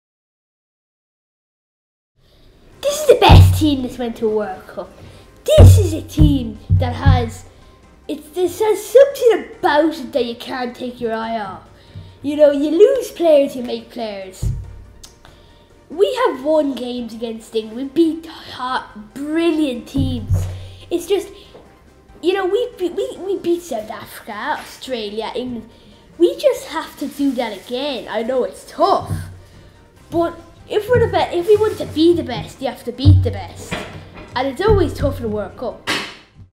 Jack Keaney auditions for minihook No.2
The prodigy sports actor Jack Keaney spills the beans on the latest sporting events for the chance to work along side his hero George Hook.